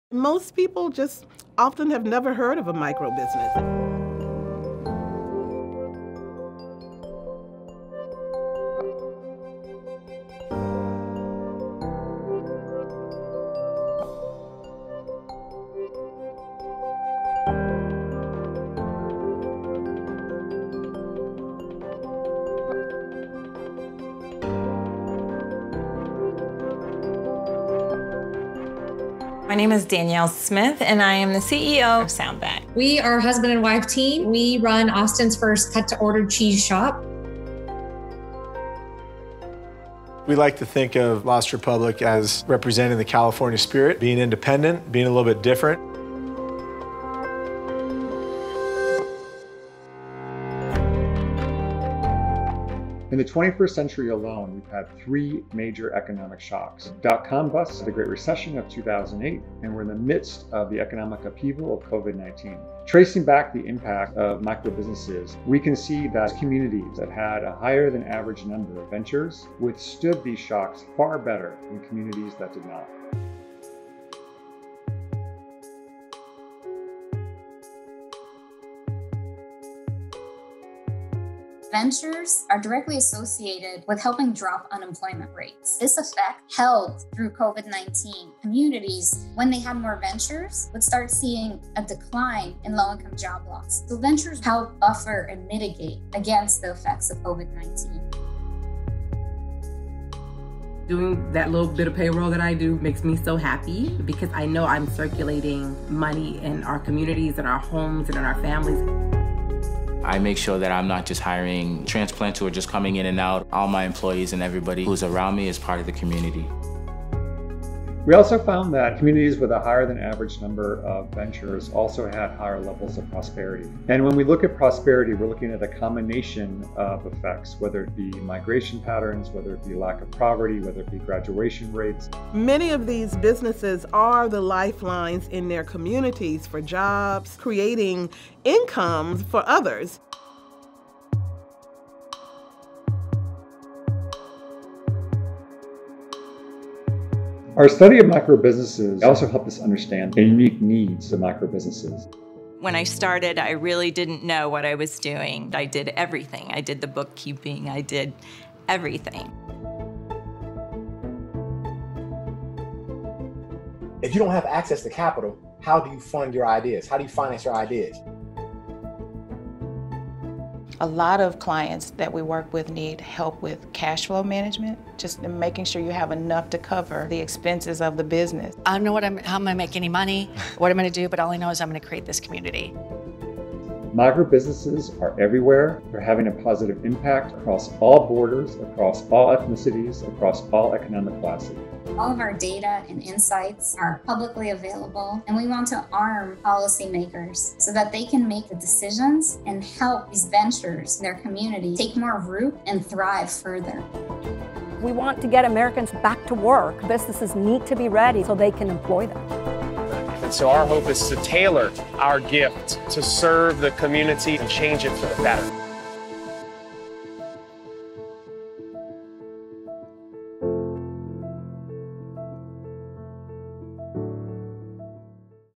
Microbusinesses: The Key to Economic Resilience in Communities
Take a look at Venture Forward, a GoDaddy study exploring how ventures help strengthen communities and mitigate against the economic effects of COVID-19.

A venture is a microbusiness, or cause with a distinct domain and an active website. Our country is filled with them— they serve as a lifeline in communities, creating jobs and income for others. Their impact on our communities has never been measured, until now. Venture Forward partnered with academic researchers to analyze 20 million GoDaddy ventures. Their positive effect on communities across the country is unmistakable. Research shows communities with a higher-than-average number of ventures withstood economic shocks far better than communities that did not— a statistic that holds amidst the economic upheaval of COVID-19. Ventures are directly associated with higher levels of prosperity and helping to drop unemployment rates. Counties with greater venture density also saw less low-income job losses. Each highly active venture per 100 people in a county increases median income by 11%. Our study also helped us understand the unique needs of microbusinesses and found that ventures need support at the local level, from access to capital to coaching and tax assistance. Explore the data and see the impact of ventures on our economy

Featured Experts:
Connie Evans, President/CEO, Association for Enterprise Opportunity
Jeremy Hartman, Vice President, Venture Forward at GoDaddy
Alexandra Rosen, Director, Venture Forward at GoDaddy
Cynthia Terry, Director of Entrepreneurship Program, Communities Unlimited